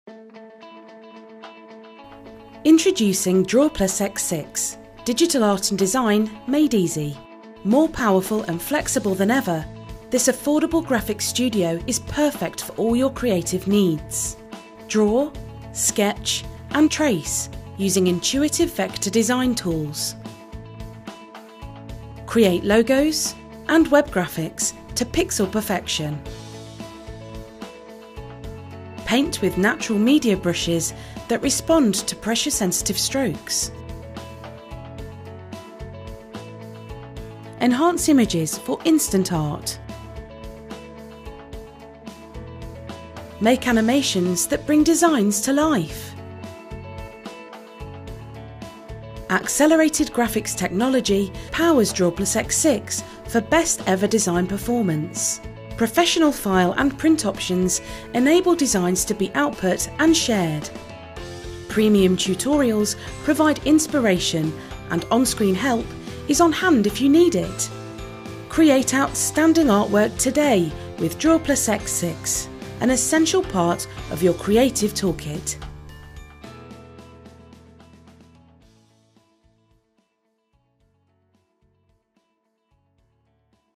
Serif DrawPlus X6 - Digital Art and Design Made Easy
DrawPlus X6 is a powerful graphics studio for illustrating, painting, and animating. Use it to design and output drawings, print documents, web graphics, images, and more.

High-end and intuitive tools make DrawPlus a great choice for professional designers and amateurs alike. It's an essential part of your creative tool kit!